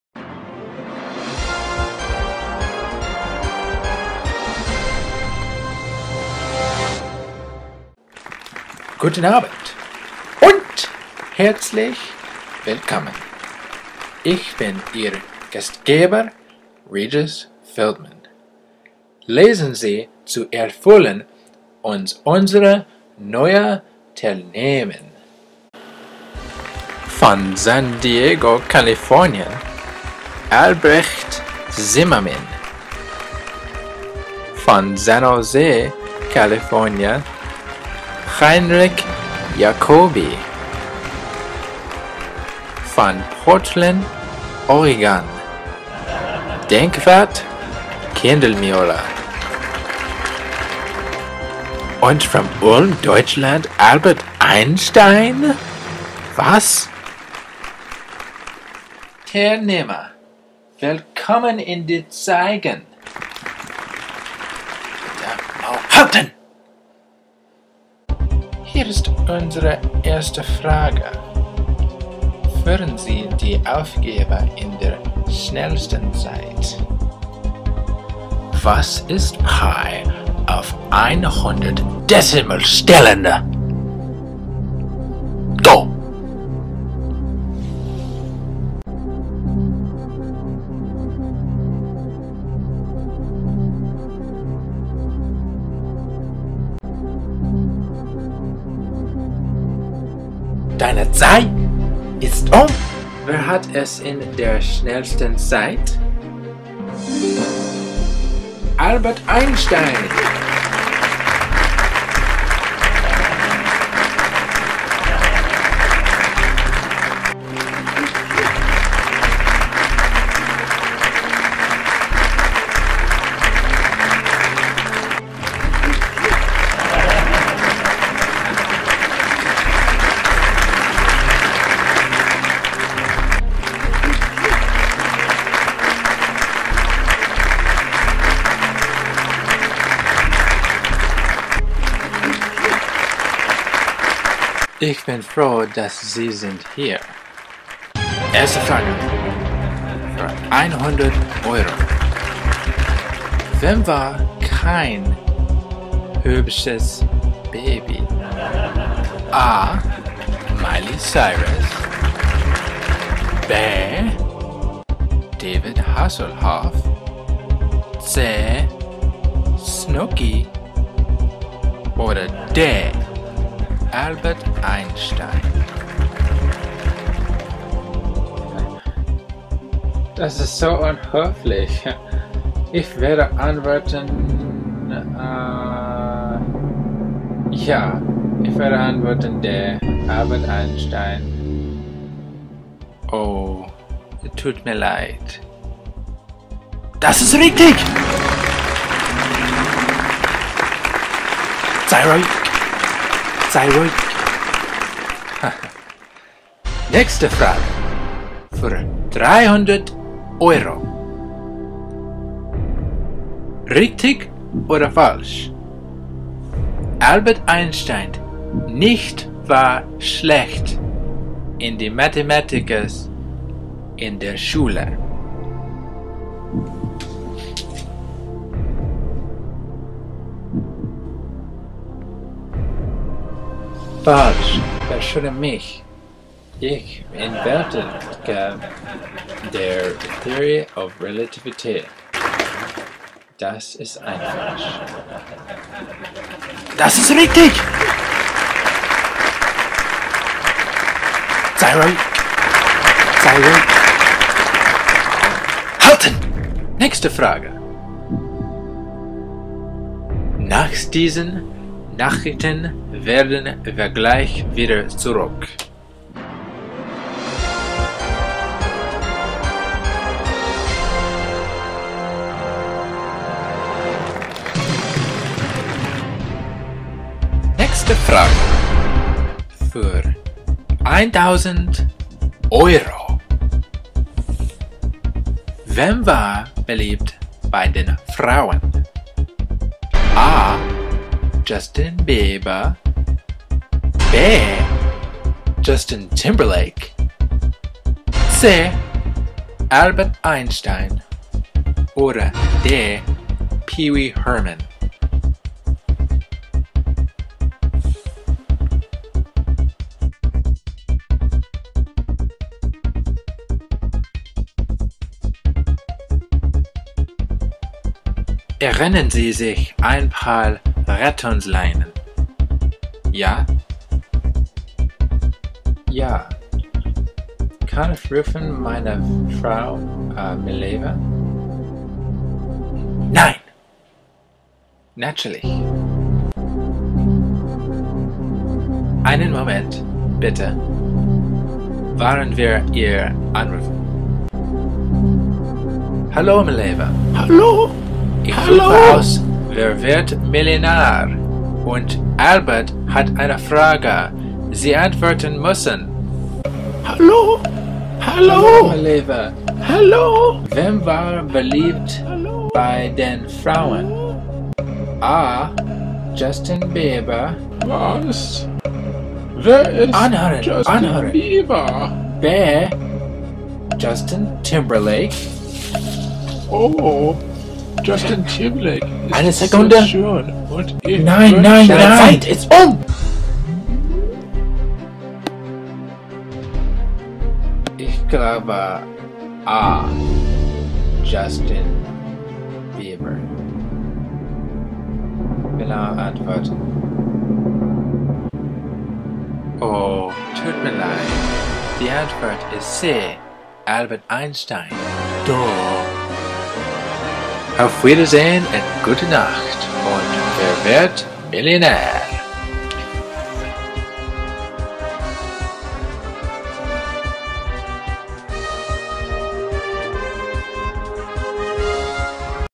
German Who Wants to be a Millionaire
Germ 201:

Might contain some errors in interpretation. My attempt to speak German and self-deprication.